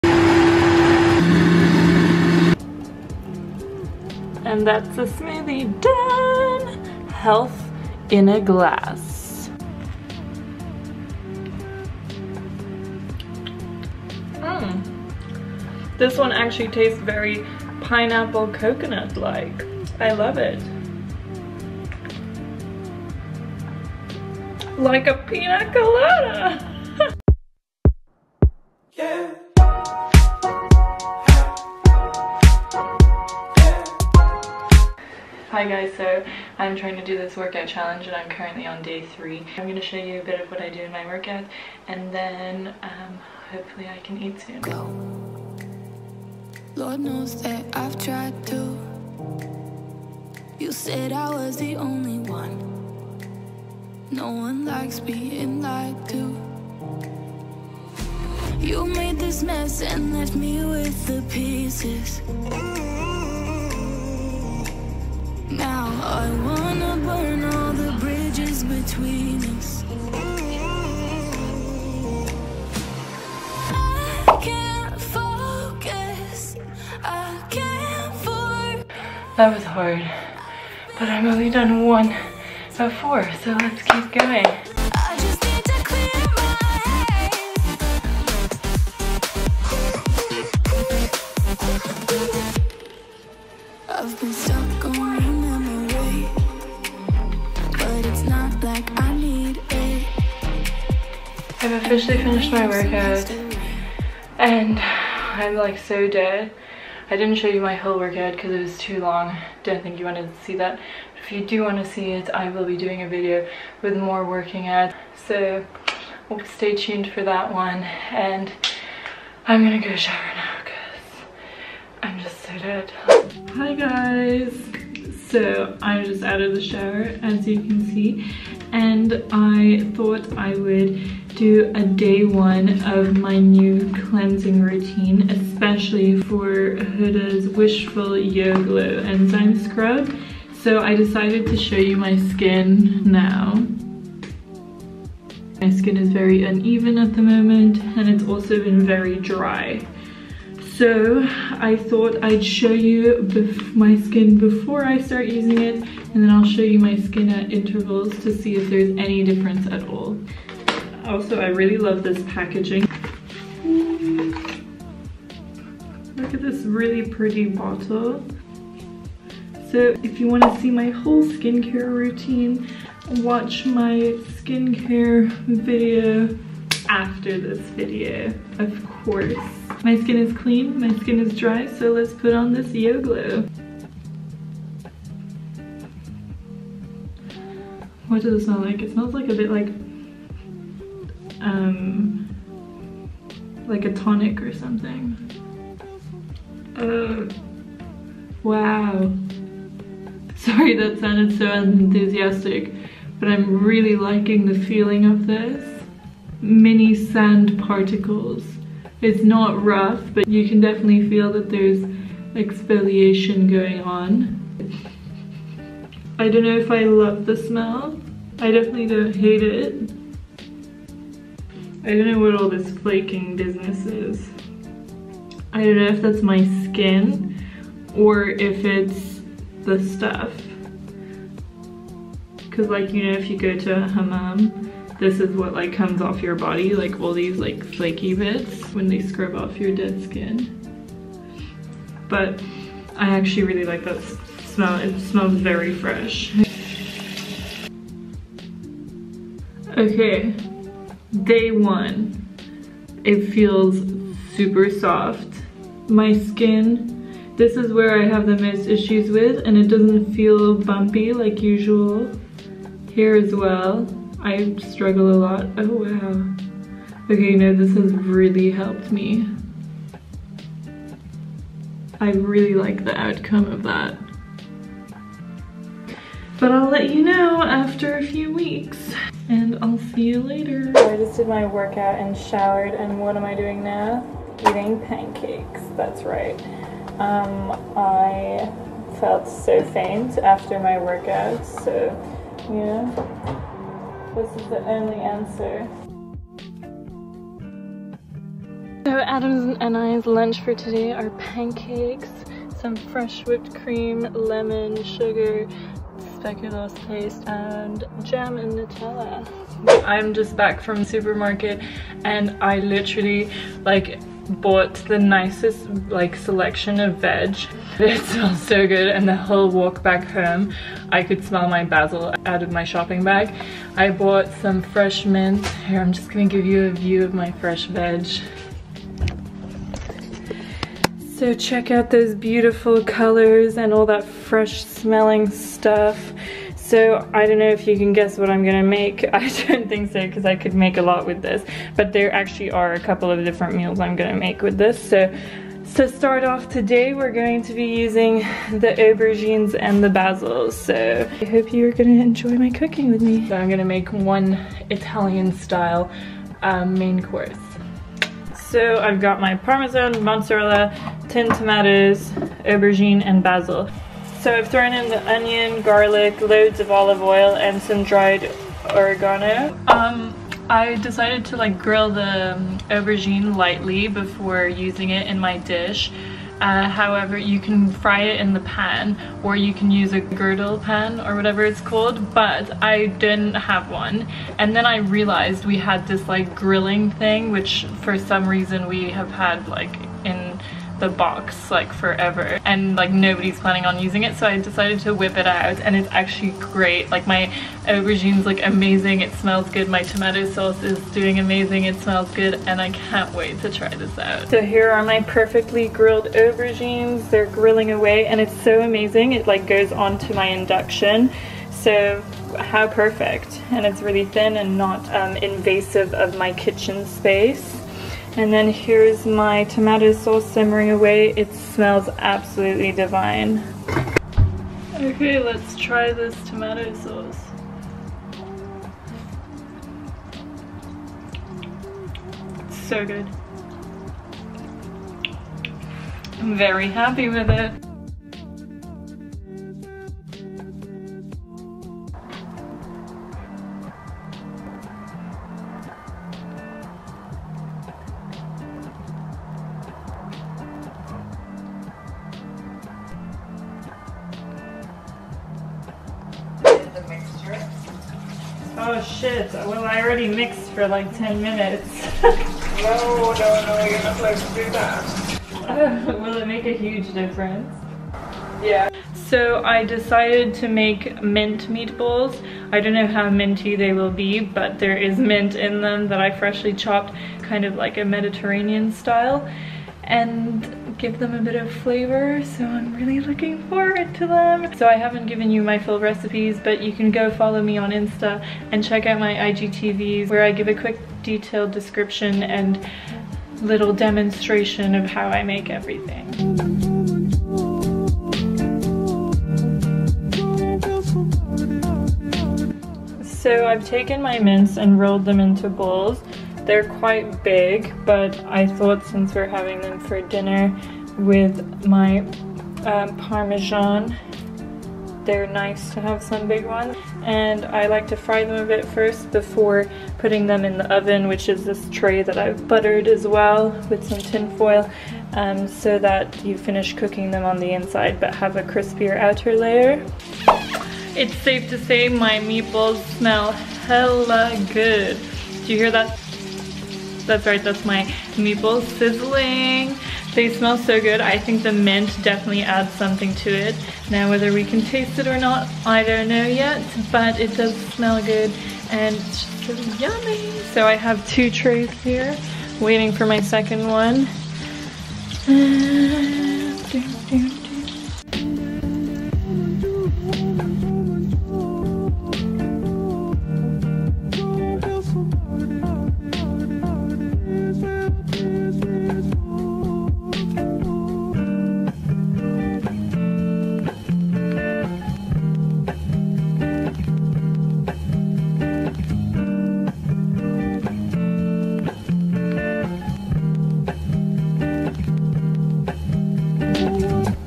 STAYING FIT WHILE EATING YUMMY *Workout, Wishful Skin Review, Italian Dinner* - Quarantine Vlog #3
Just another quarantine vlog to keep you entertained. I found it very tricky to stay motivated to workout in the beginning of the lockdown but recently have gotten on track and am gonna try sticking to it. You can see some of the workouts I'm doing as part of my 2 week shred which will maybe give you inspiration to start too! Who knows.. maybe we'll need a summer body after all! I haven't been watching what I eat but because I'm always cooking I find I'm keeping myself busy and don't have as much time to snack, as well as the fact that I'm trying to be slightly healthy so I won't have to be rolled out of quarantine!

Huda Beauty released a new line of skincare called Wishful Skin and one of my many quarantine online purchases has been the Yo Glow Enzyme Scrub. I thought I'd review it for you.. and tbh so far so good!! Also the smell lasts throughout the day and I love it.

For dinner I made an aubergine parmigiana and meatballs for an Italian dinner which was very successful! Watch me cook and get inspired!

Hope you like this stayhome

I will be posting a video in 2 weeks of the fitness challenge I am trying to complete so make sure you keep an eye out for that!

Stay safe & healthy x